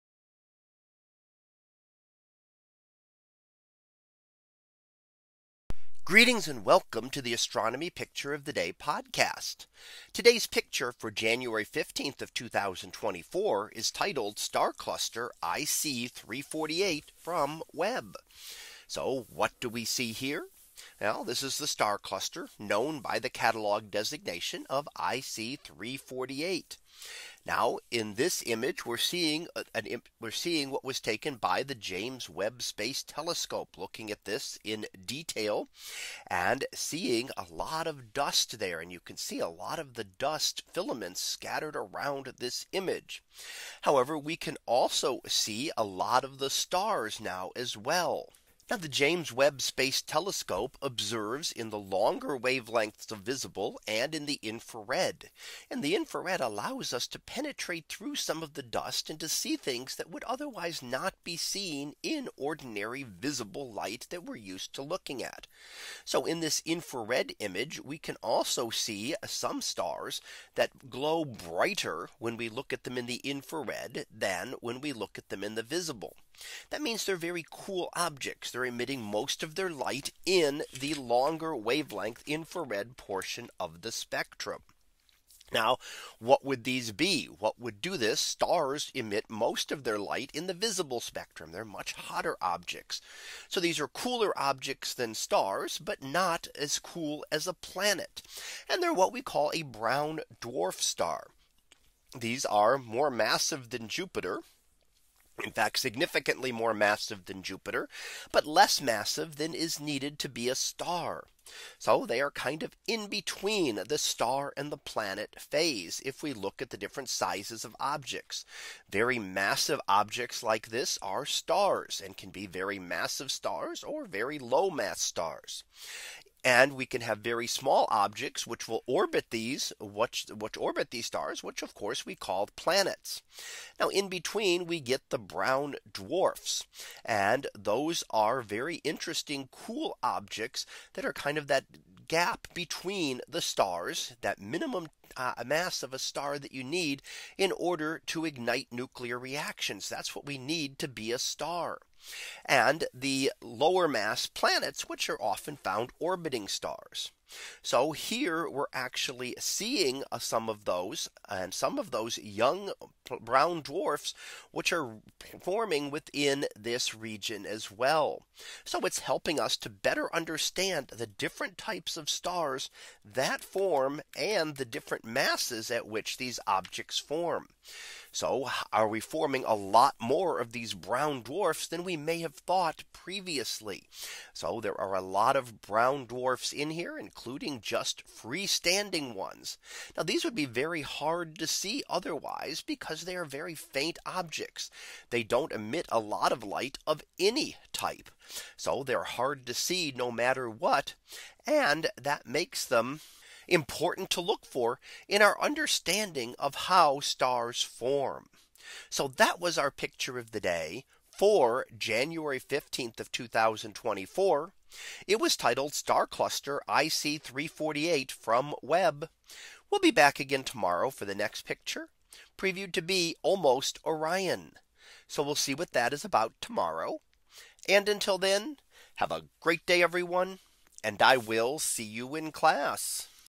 2024 January 15 - Star Cluster IC 348 from Webb
In today's image, we see the star cluster known by its catalog designation of IC 348. Many young clusters like this are hidden by dust and not well studied in visible light. The James Webb Space Telescope which also observes in the infrared gives us a look into this young cluster, including the many brown dwarf stars forming here.